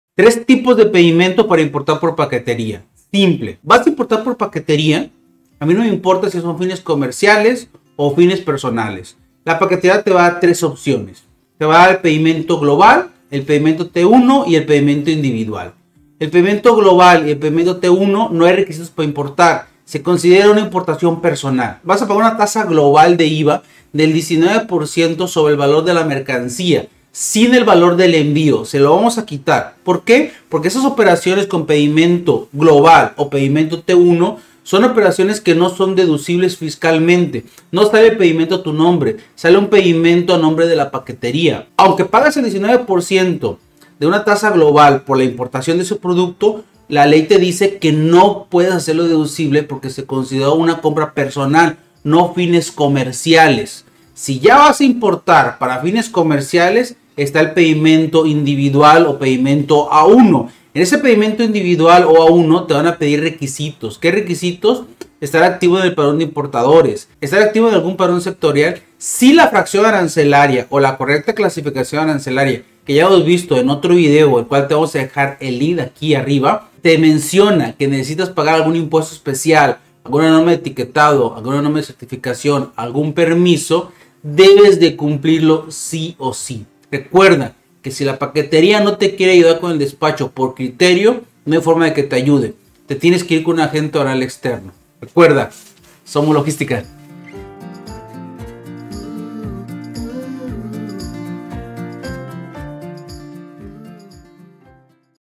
IMPORTACIONES PAQUETERIAS | PEDIMENTO GLOBAL | PEDIMENTO T1 | PEDIMENTO INDIVIDUAL |DEDUCIBLE FISCAL
Hoy hablamos sobre un tema que muy frecuentemente me pregunta y es acerca de los pedimentos.
La paquetería a la hora de importar te lo puede hacer por varios tipos de pedimentos, en el video te explicamos algunos, cuales son sus diferencias y cual te conviene mas dependiendo la importación que vas a realizar

00:00 Importación Por Paquetería
00:09 Tipos de Pedimentos
00:15 Pedimento Global o T1
00:53 Pedimento Individual o A1